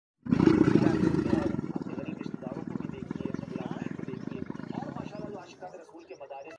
Camel sounds
Camel breeding sounds for mating